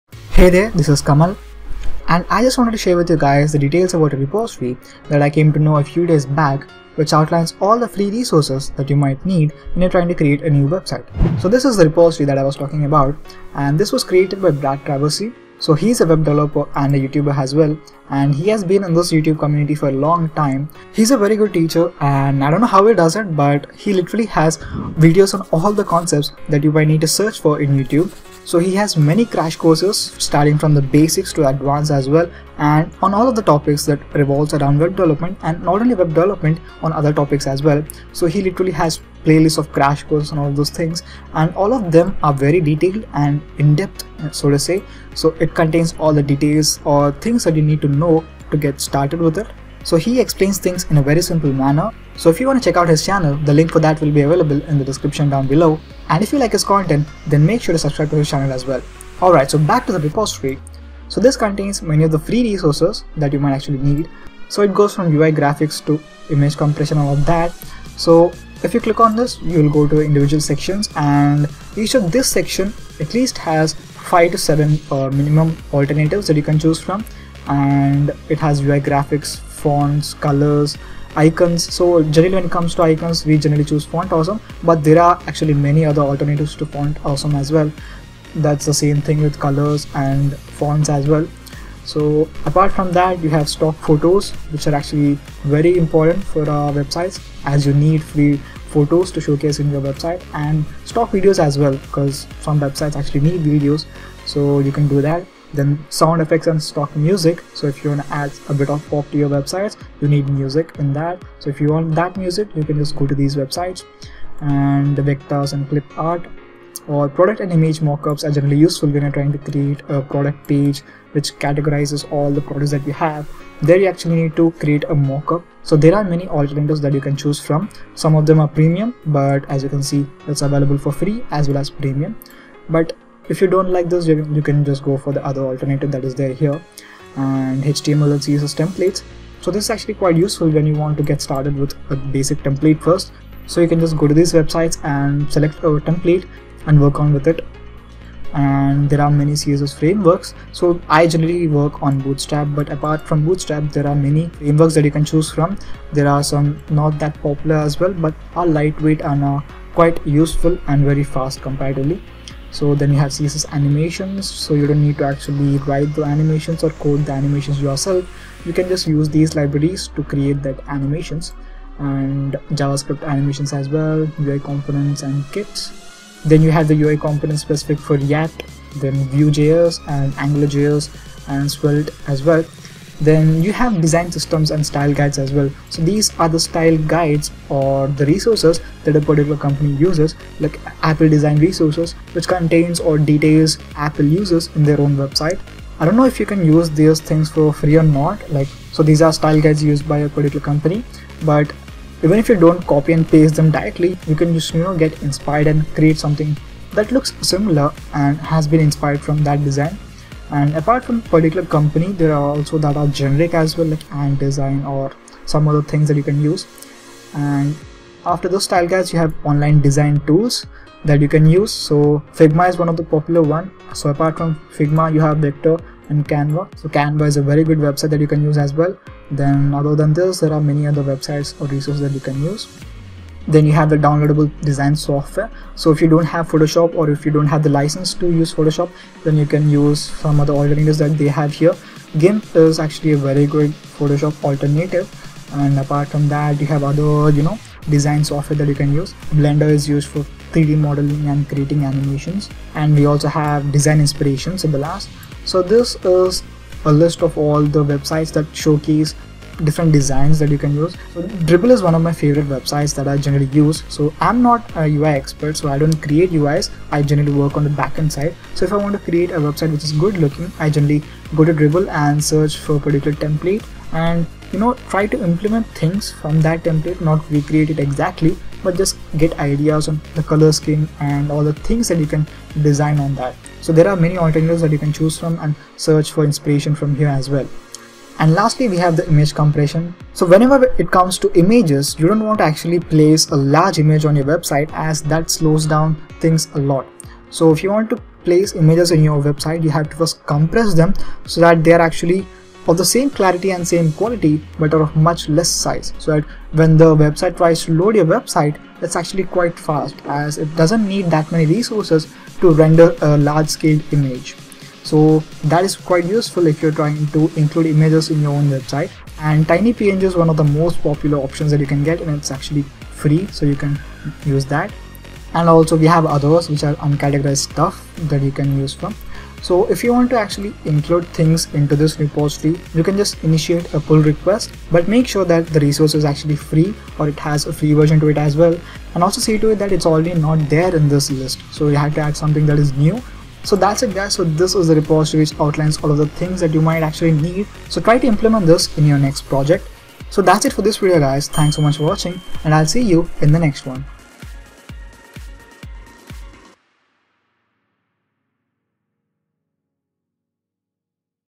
Free Design Resources for Web Developers
This repository is created by Brad Traversy. Check out his channel to get to know more about his content.